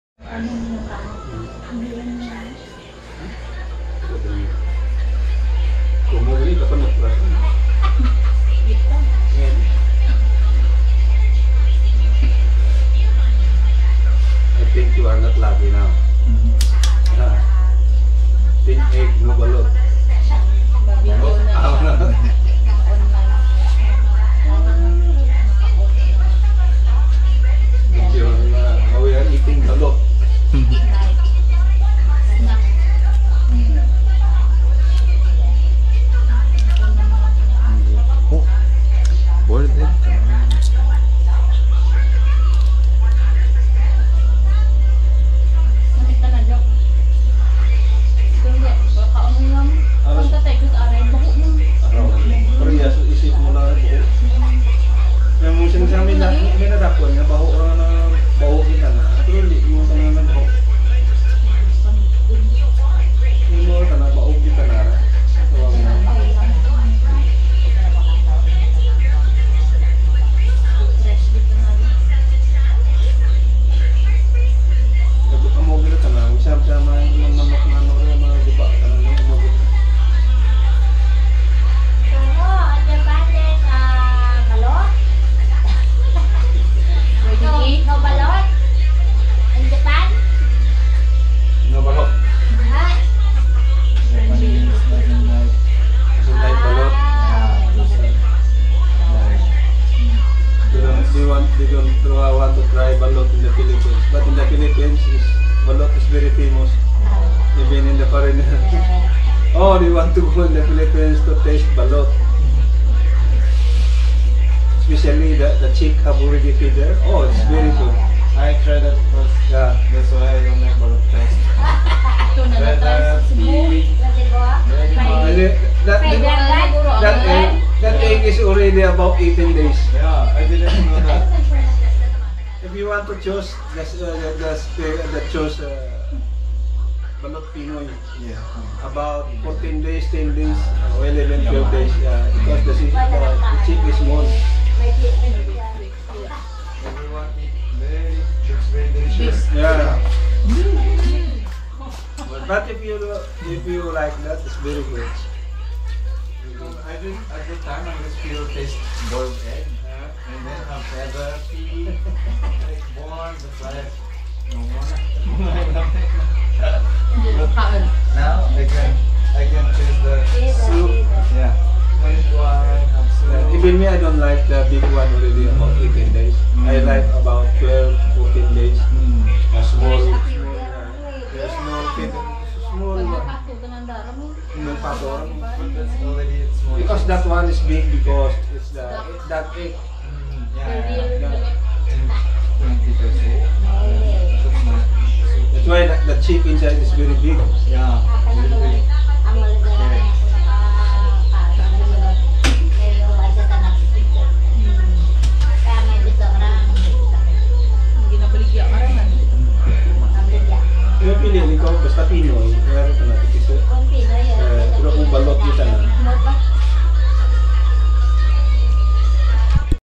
【日常】夜食にみんなでバロットを食べてみた
孵化装置の中の卵が有精卵でなかったり
孵化まで育たなかった卵は
義父がゆでてみんなで食べます。

味はゆで卵ですが、中にスープがあったり
食感が全然違って美味しいです。

ぜひフィリピンへ来た際は！

ご意見、ご感想、改善点などがありましたら
ぜひぜひコメントしてください！